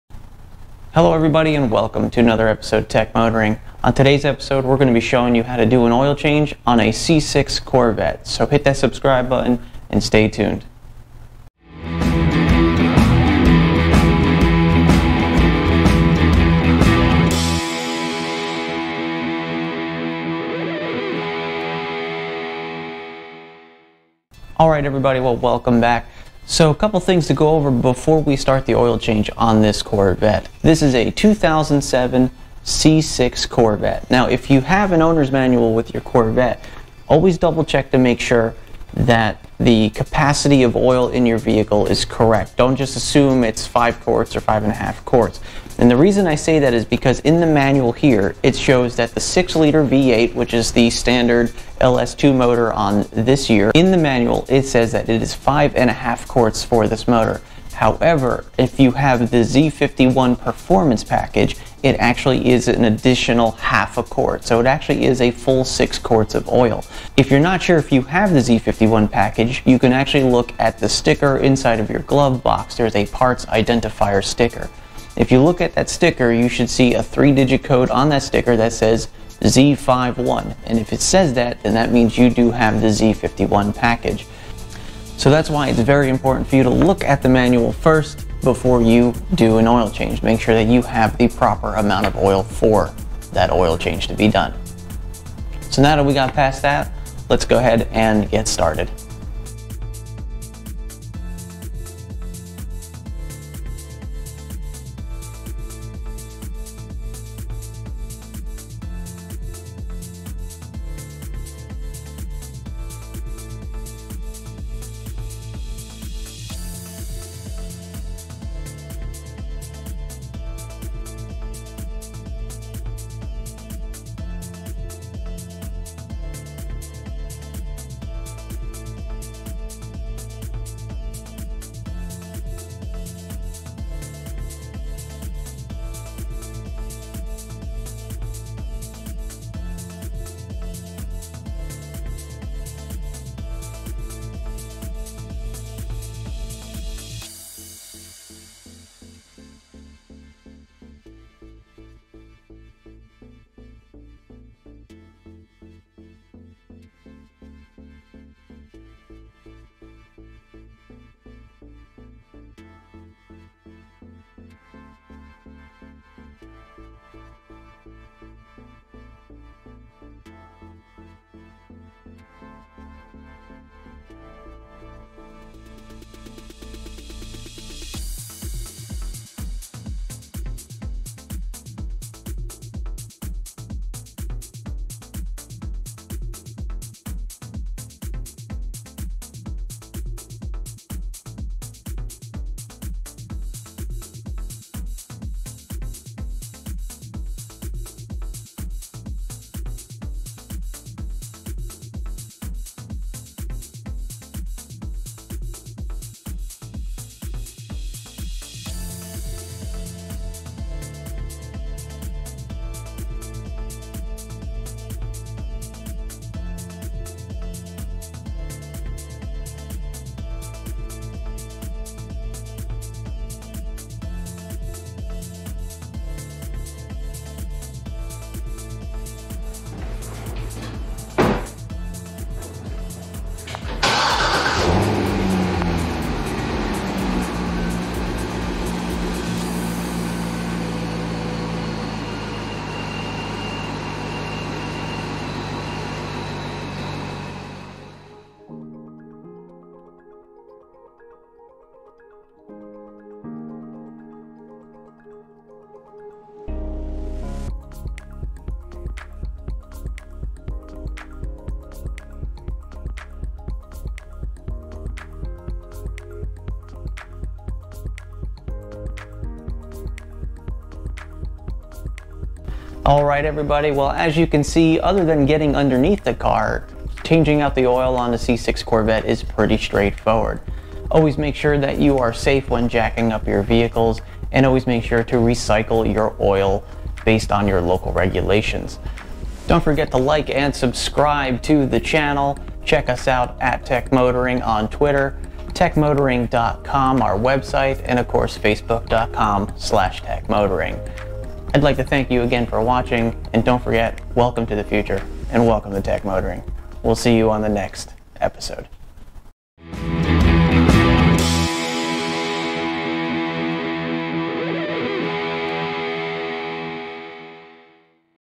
Change Your Own Oil - C6 Chevrolet Corvette
In this episode we show you how easy it is to change your oil on a 2007 C6 Chevrolet Corvette.

Keep in mind that the process for a Z06 is different. Only use this guide for the 6.0L motor.

This guide assumes you understand the basics of working on a vehicle, and the risks associated with it.
Always use caution when doing any maintenance on a vehicle. If you are uncomfortable with any step of the maintenance please refer to a professional for help.
If you need to lift your vehicle in order to do any work, make sure to use the proper jacks and jack stands to support the vehicle.
Please remember to recycle your oil based on you local regulations.